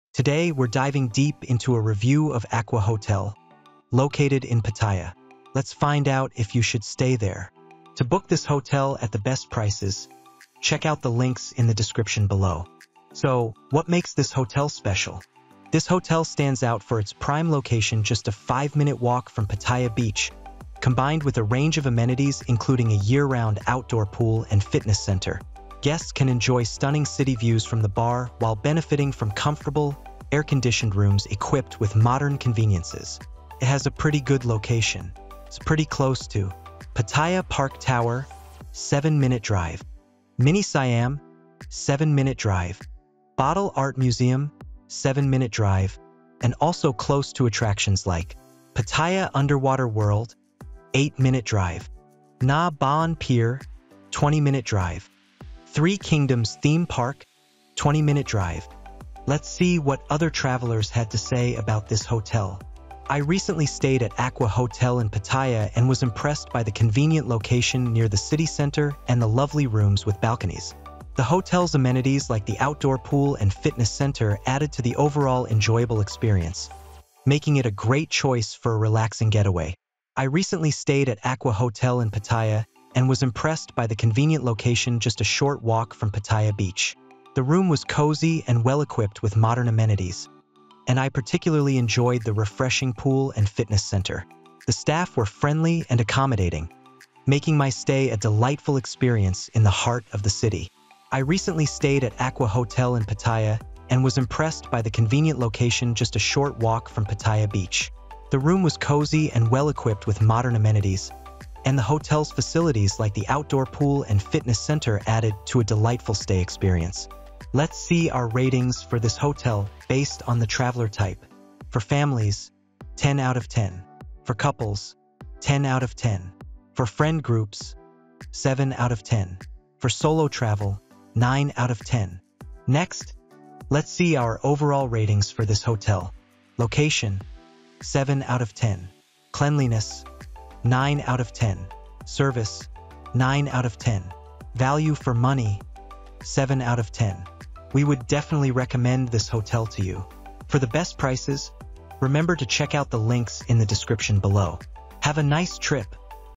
Acqua Hotel, Pattaya Review - Is This Hotel Worth It?
In this video, we reviewed the Acqua Hotel, Pattaya based on its price, quality, value for money, and more. We also gave it ratings based on the travel type and more.

Additionally, we'll provide you with insights into the nearby attractions and activities that you can enjoy during your stay at Acqua Hotel, Pattaya. Whether it's exploring the local culture, sightseeing, or shopping, we've got you covered.